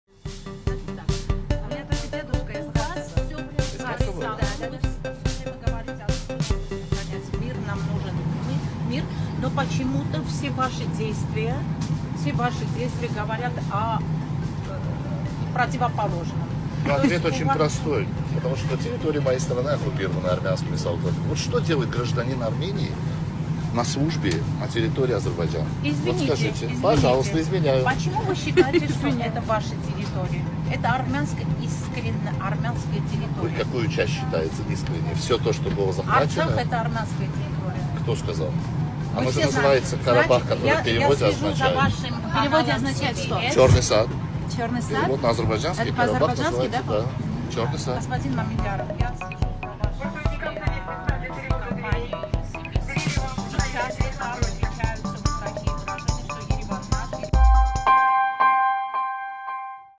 Elmar Məmmədyarov erməni jurnalistə Qarabağ sözünün mənasını başa salır
Brüsselə səfər zamanı erməni jurnalistin suallarına tuş gələn Xarici İşlər naziri suallara ayaqüstü cavab verib.